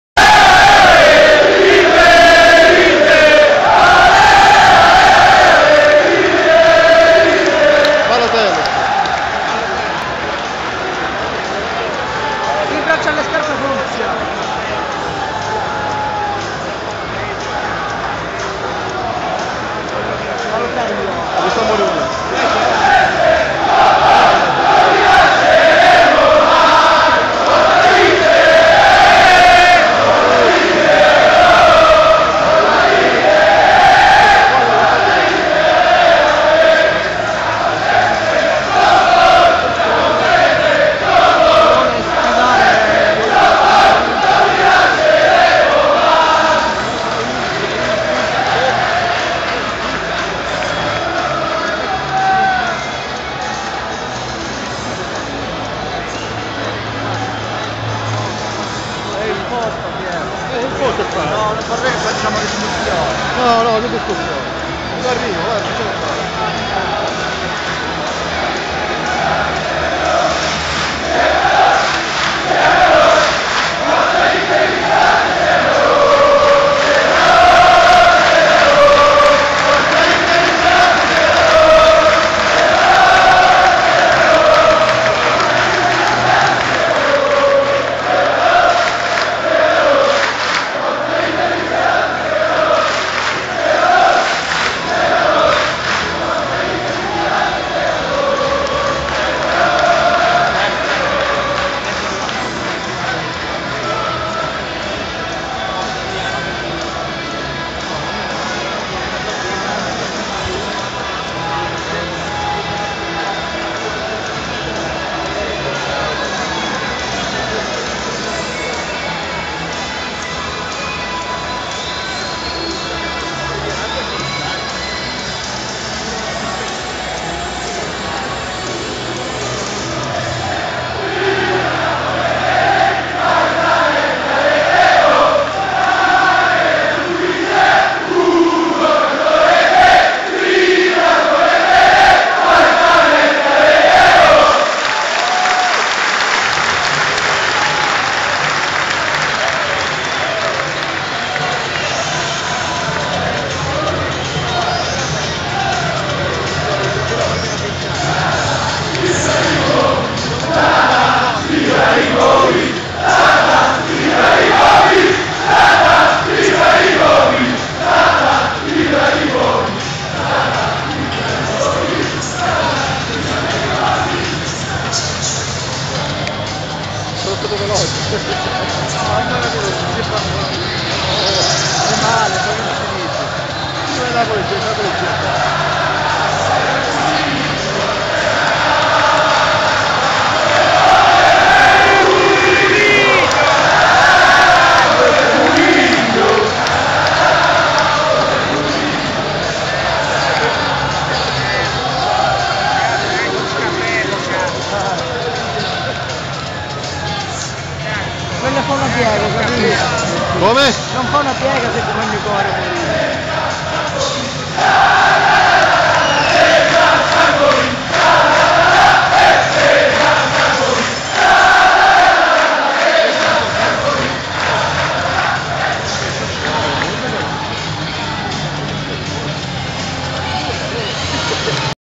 Manchester United-Inter - Riscaldamento dell'Inter
Dall'old trafford di Manchester,lo United sfida l'inter nel ritorno degli ottavi di finale della champions league!!!Questo video mette in mostra il riscaldamento della squadra di mourinho con i cori rivolti a tutti i giocatori dell'inter!!!